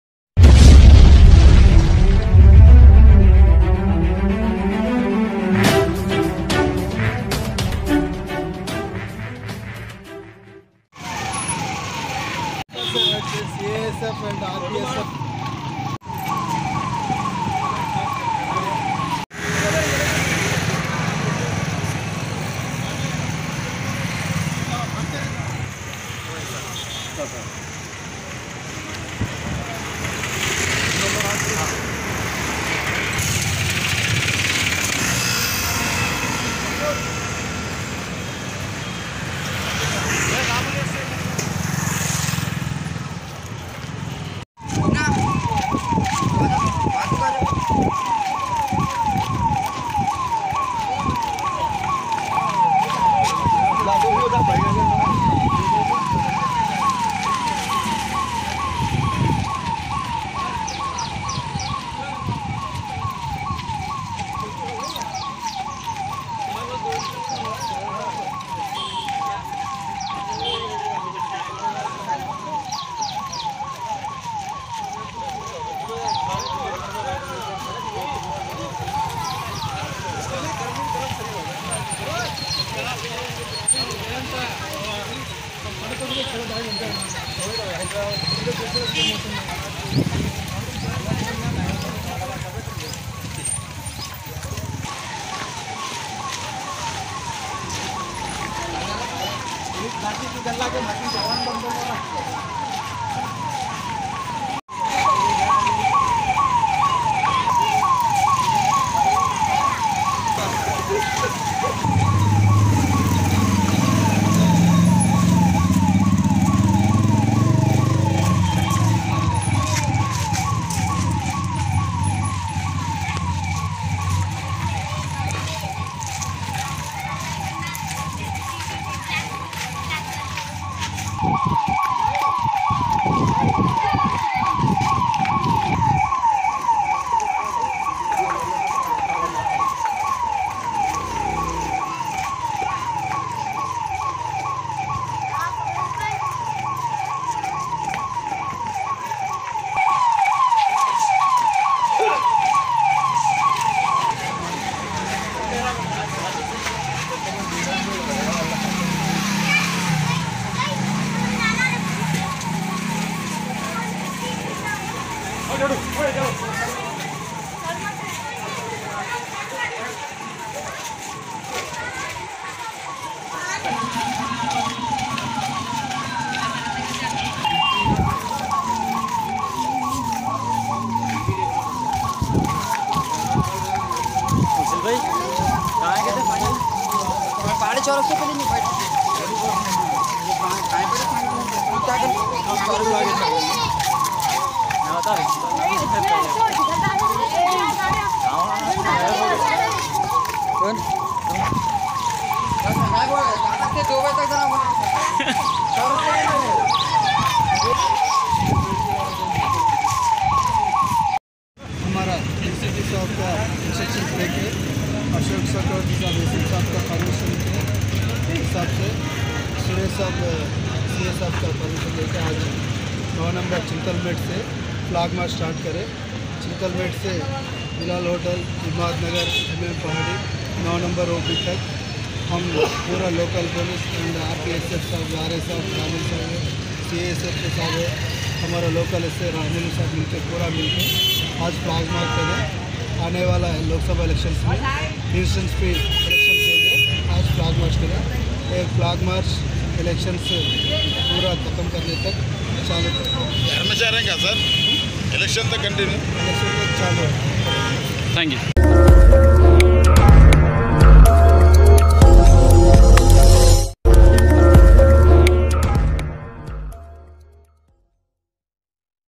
Today Rajendra Nagar flag March Chintal mate Suleman Nagar Imad Nagar mm Pahadi 9 number.
*Today Rajendra Nagar flag March Chintal mate Suleman Nagar Imad Nagar mm Pahadi 9 number..!! ||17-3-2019|| HYDERABAD || REPORT BY : MOHAMMED SHAHBAZ. ||CHIEF JOURNALIST OF RIGHT_MEDIA_NEWS||.*